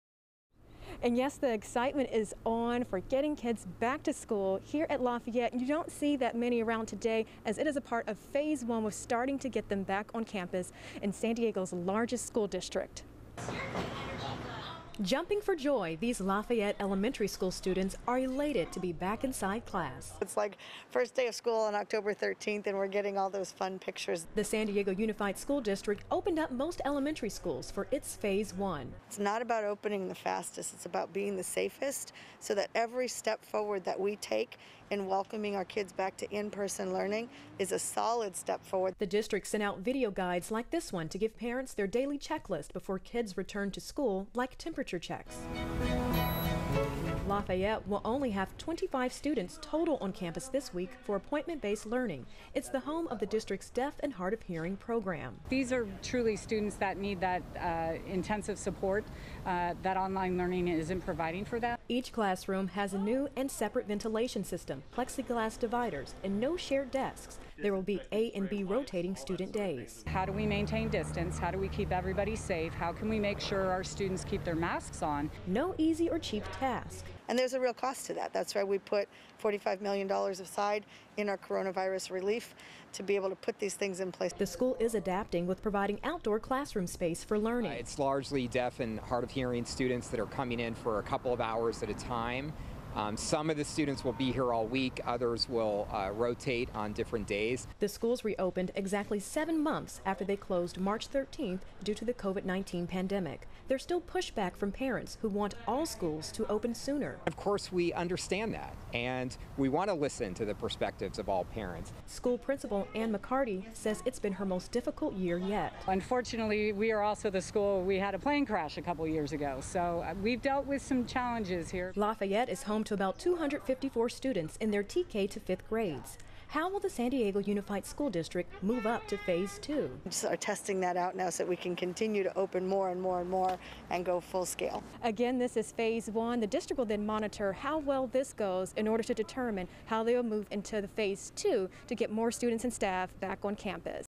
San Diego Unified School District reopens some elementary schools for phase 1
In-classroom learning was back in session Tuesday for many elementary schools in the San Diego Unified School District as part of the district's phase one plan.